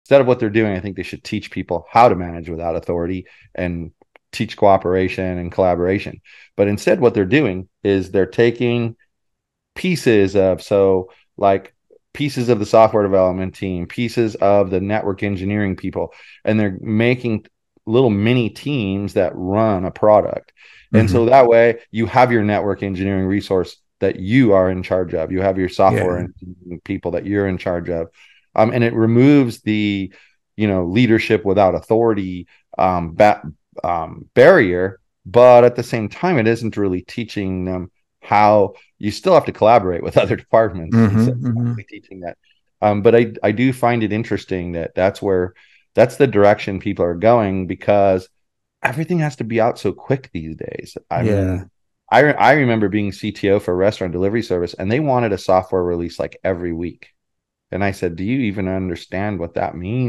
Unleashing the Power of Mini Dream Teams: Breaking Down Silos in Software Development
Embracing a new era of collaboration where network and software engineers unite!

Yes, we face the rush of rapid releases, but who said innovation wasn't a wild ride? Software development will never be the same!